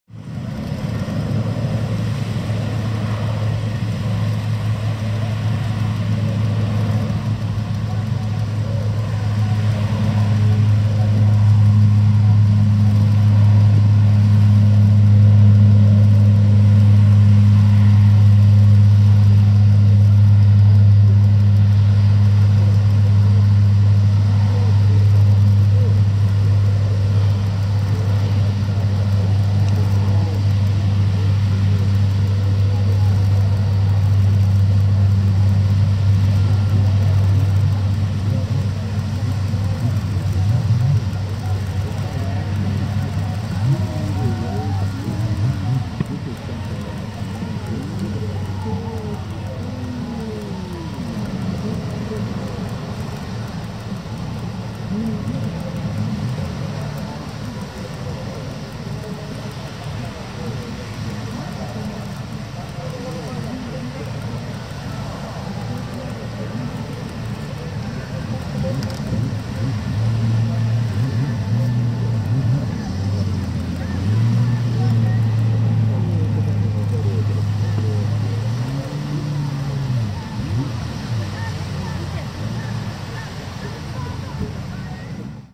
海上自衛隊 飛行艇 US-2 東京湾への着水シーン 2019/9/8
2019/9/8 レッドブルエアレース 2日目、
習志野親水護岸の先より撮影。
昔、船の科学館に展示されていた「二式大艇」が思い起こされます。